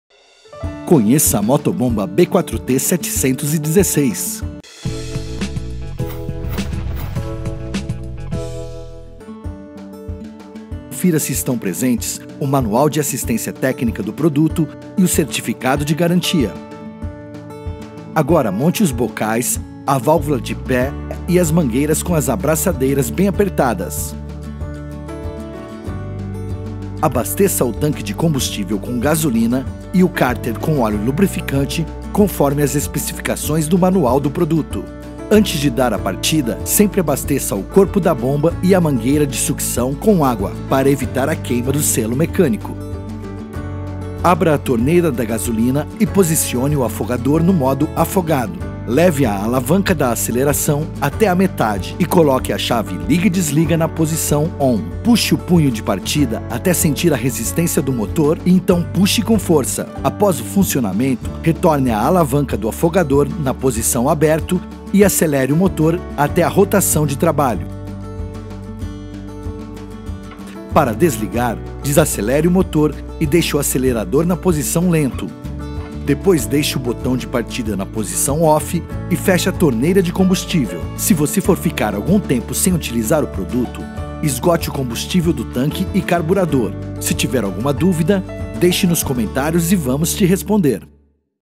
Branco Explica - Motobomba B4T-716
Precisa transferir água de um lugar para outro? Então, conheça tudo sobre a Motobomba B4T-716. Ela é indicada para uso em pequenas irrigações e construção civil, deixando seu trabalho mais prático e rápido.

Assista a mais um vídeo da série Branco Explica e usufrua ao máximo do seu equipamento!

E caso ainda tenha alguma dúvida técnica sobre a Motobomba B4T-716, confira